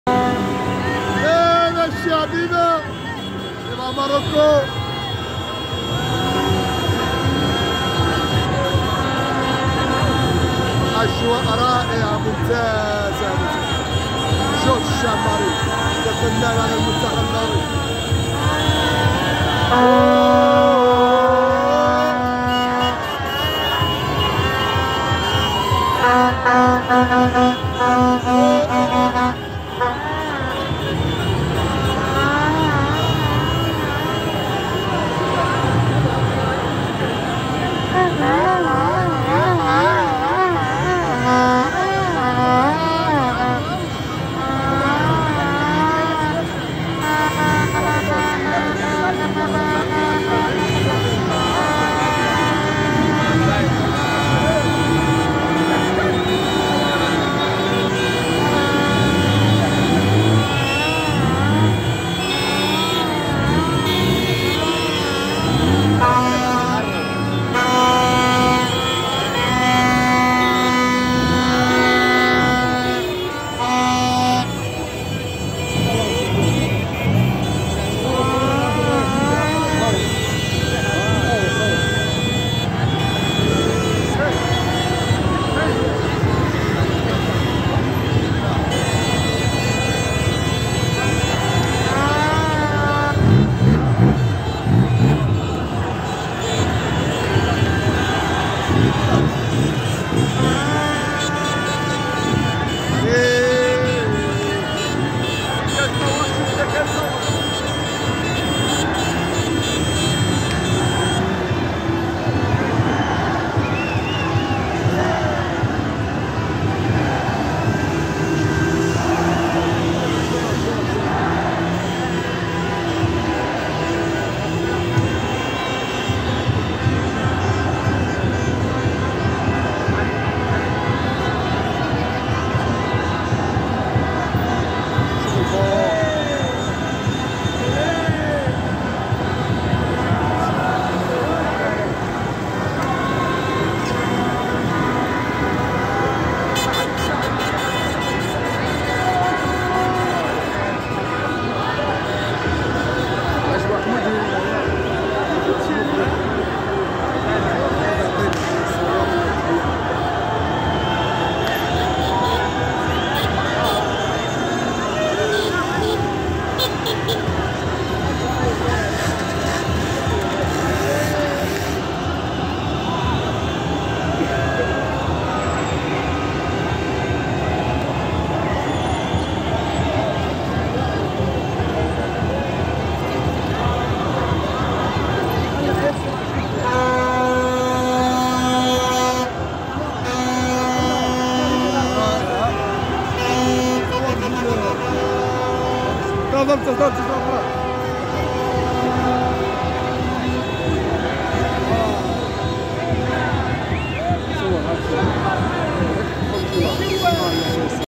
7 décembre 2022
شعب مغرب مدينة طنجة فرح المنتخب المغربي